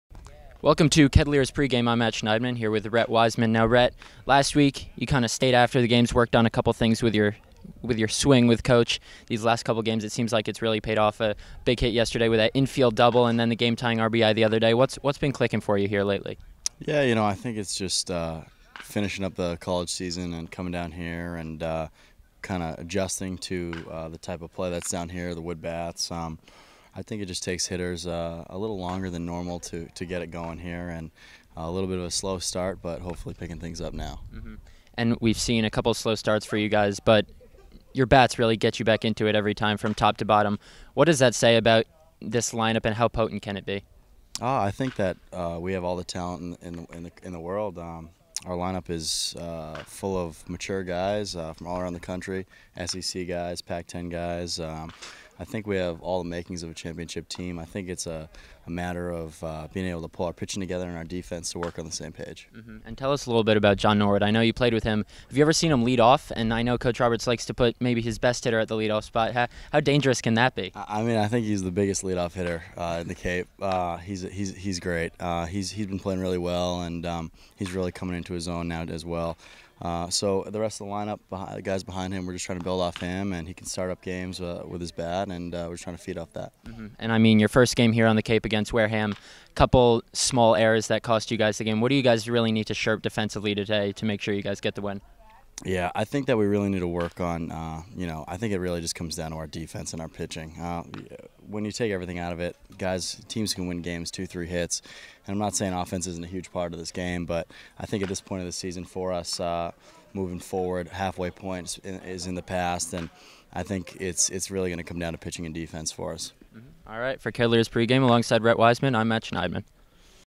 Kettleers Pregame w/ Matt Schneidman
Matt Schneidman talks with RF Rhett Wiseman (Vanderbilt) ahead of Saturday's game against Wareham.